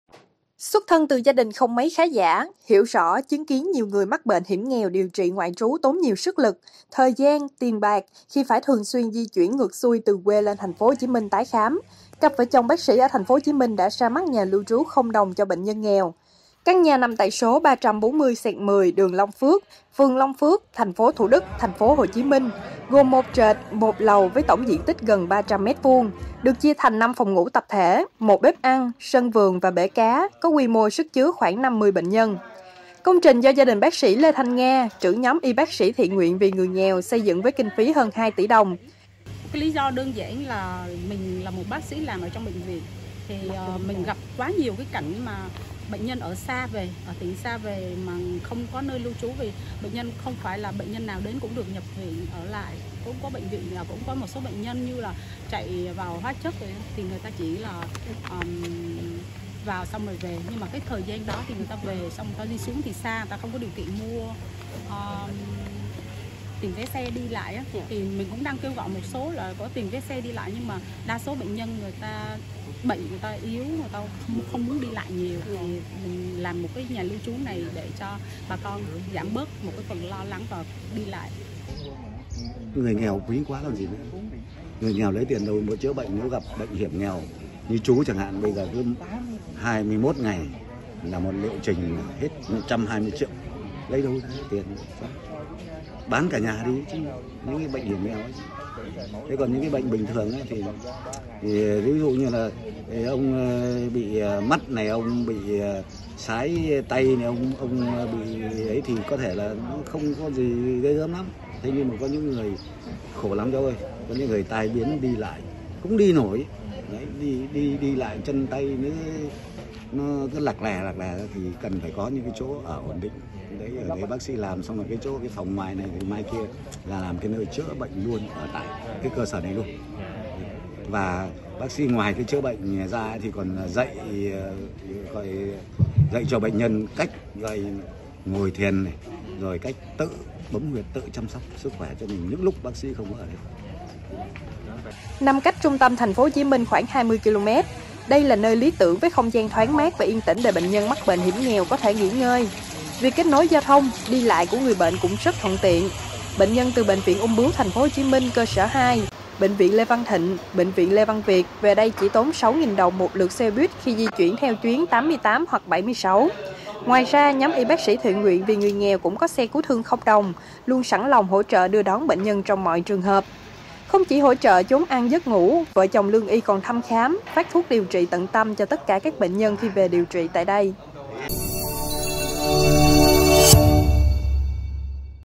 Bác sĩ xây nhà lưu trú 0 đồng cho bệnh nhân nghèo ở TP.HCM
Gia đình bác sĩ Lê Thanh Nga dành 2 tỷ đồng xây nhà lưu trú 0 đồng với diện tích 300m2, phục vụ cho bệnh nhân nghèo đến TP.HCM khám chữa bệnh.